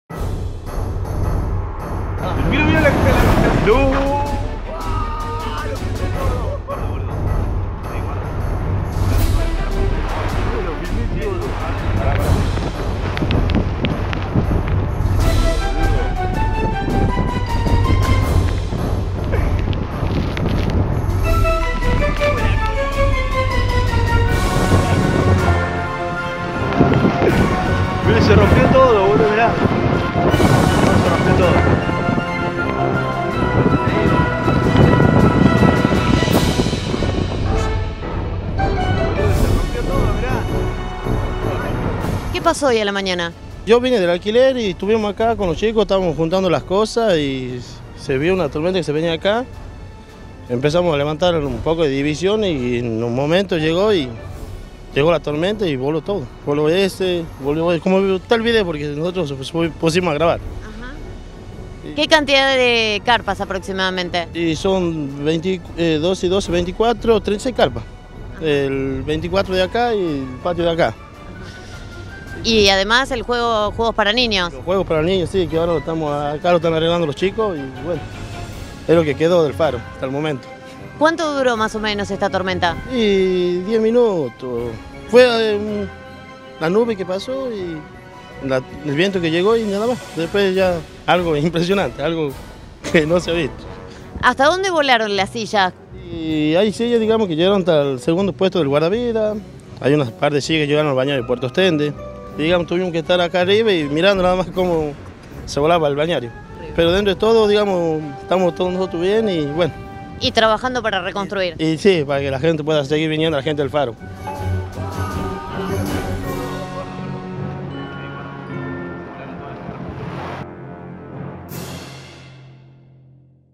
TEMPORAL EN PINAMAR: SE VOLÓ TODO
Víctor Sosa, empleado del Balneario El Faro relata cómo el temporal hizo que en tan sólo 10 minutos se volaran 36 carpas. Asegura que “llegó la tormenta y voló todo”.